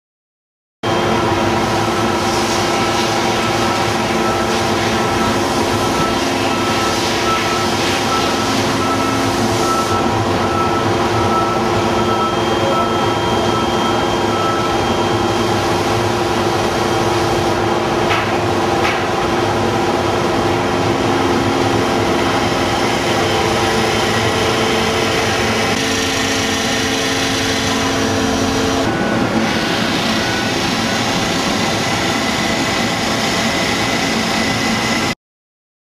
Robotic Ladlers, Advance Automation from Industrial Innovations
Featuring a servo motor and drive, Advance Automation Ladlers are precise and rely upon Industrial Innovations proprietary Smart Probe™ ladle system circuitry to prevent dunking.